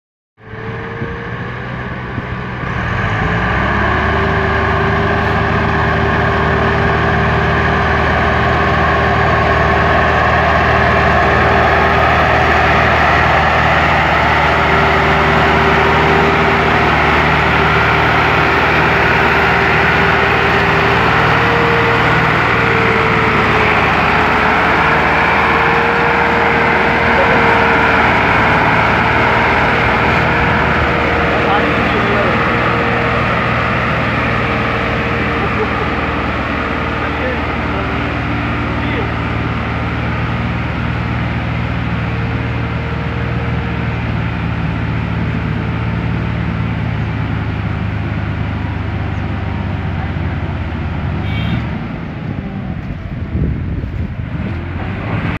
Massey fergusson 385 helping mf260 turbo.
Mf 260&385 pull Troly in mud
Qazi Ahmad ali
Qazi farm kasur